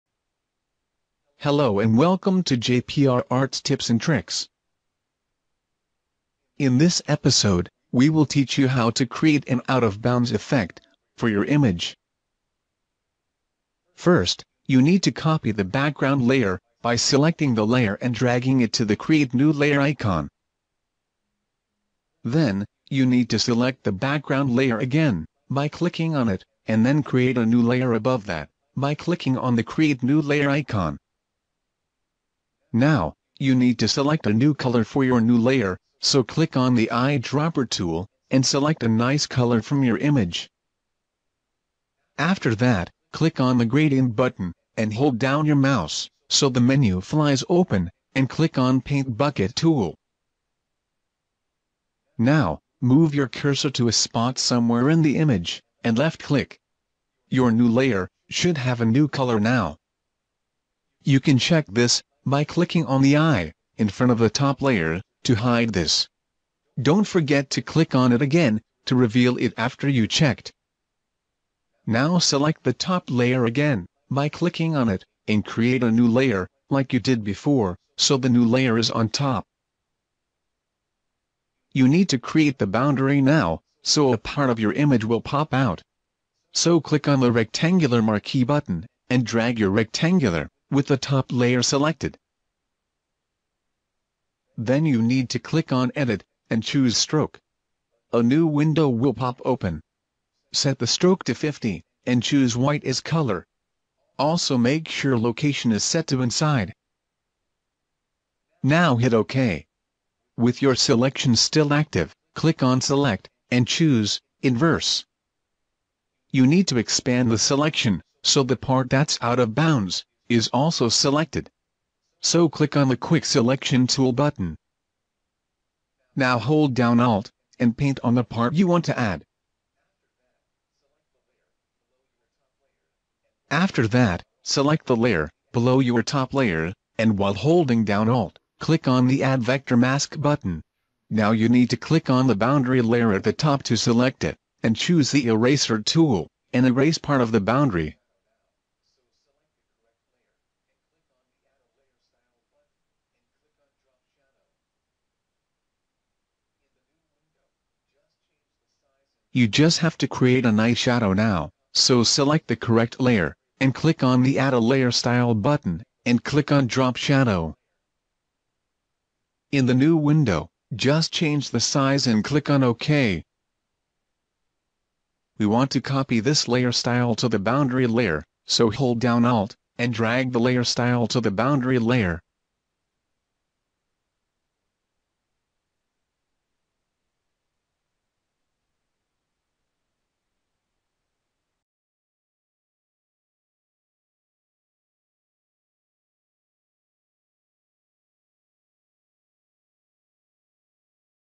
How to create an out of the frame effect for your image using Adobe Photoshop.
Learn in just a few steps, how to create a cool looking out of bounds effect on an image.